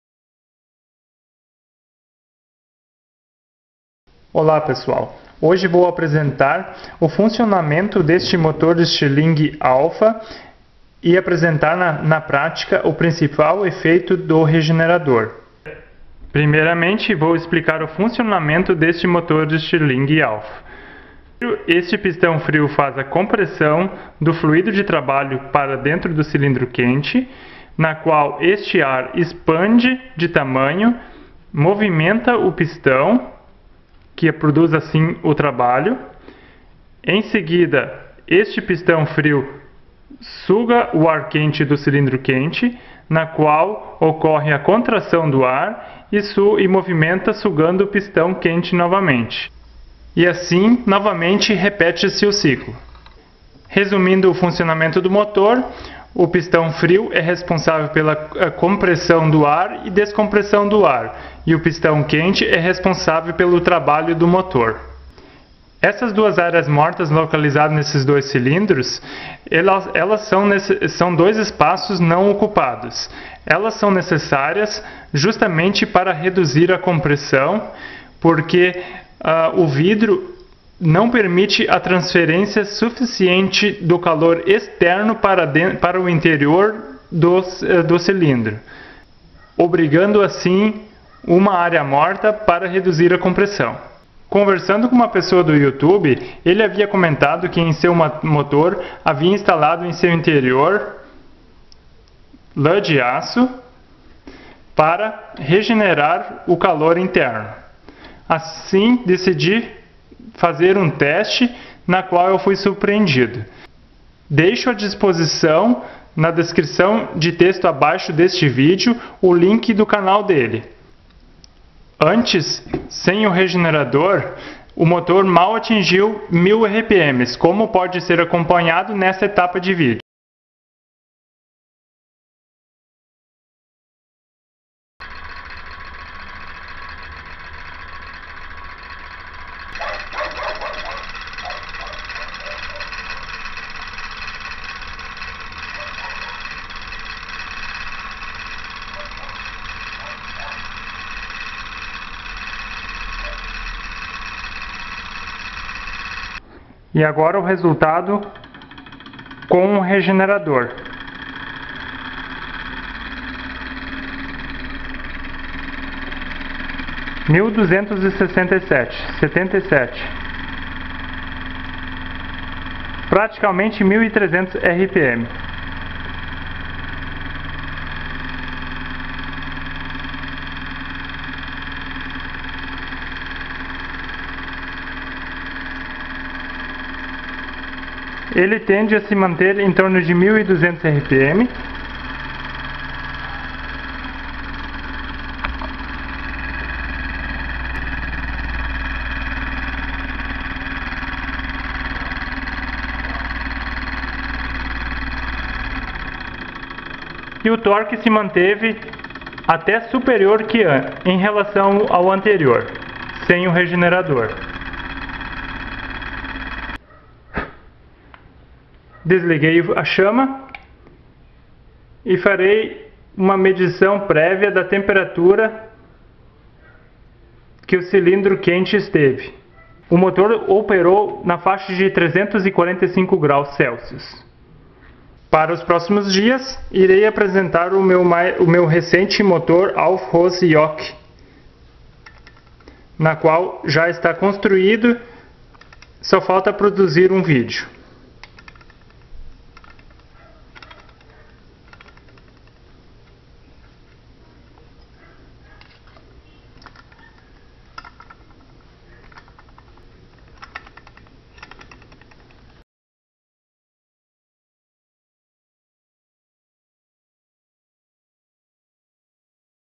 Como funciona um motor Stirling Alfa e com regenerador - how it works a Stirling engine Alfa
Este vídeo explica funcionamento de um motor Stirling Alfa e apresentei na prática, a principal vantagem de um regenerador.
O motor Alfa, possui um funcionamento diferenciado em relação ao modelo Gama, apesar da grande semelhança entre os dois modelos. A principal característica que diferencia o motor Alfa do Gama, está descrito nessa página:

No motor Alfa, o pistão frio somente é responsável pela compressão e descompressão do sistema, e o pistão quente é responsável em produzir o trabalho do motor.

Como pode ser visto nos instantes iniciais do vídeo, pistão frio faz a compressão do fluído de trabalho para o interior câmara quente, neste caso seria o ar, em seguida, esse fluído de trabalho, expande dentro do cilindro quente, impulsionando o movimento do pistão quente.

Logo após, o pistão frio suga o fluído de trabalho do cilindro quente, ocorrendo a redução do volume do ar, sugando o pistão quente, em seguida repete-se novamente o ciclo.

Nesse projeto de motor, foi necessário essa área morta, um espaço não ocupado, para reduzir o efeito da compressão, essa compressão seria benéfica, desde que o vidro do cilindro quente, permitisse a maior transferência de calor para o interior do cilindro, como o vidro é um péssimo condutor térmico, foi necessário projetar essa área morta.

Como funciona um regenerador? O regenerador, absorve uma parte do calor que seria rejeitado pelo motor, e reaquece o ar frio ao entrar dentro do cilindro quente, proporcionando maior disposição de calor dentro do cilindro quente. Um regenerador, tem um funcionamento muito parecido como uma bateria, porém ele é um acumulador térmico.

Conversando com o amigo "grant31781" ele comentou que havia acrescentado dentro dessa área morta, um regenerador feito com lã de aço. Então decidi fazer o teste e o resultado foi muito bom, o motor passou dos 1020 rpm, para os atuais 1295 rpm, somente acrescentando no motor, o regenerador.

Agradeço a atenção de todos.
Leandro Wagner.